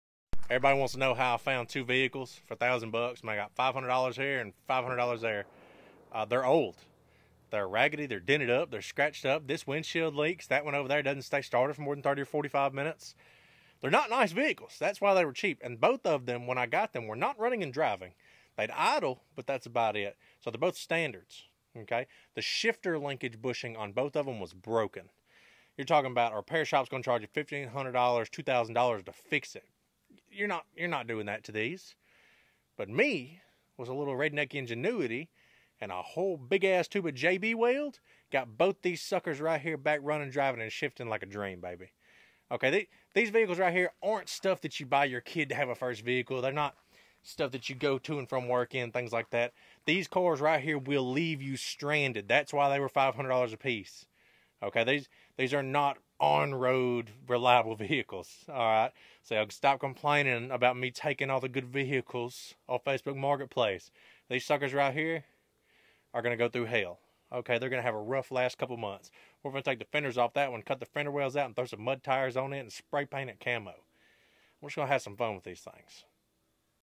Two $500 vehicles! I find the best deals because I like working on stuff!
Gonna turn these two bugs into who knows what! I’m open to all ideas!!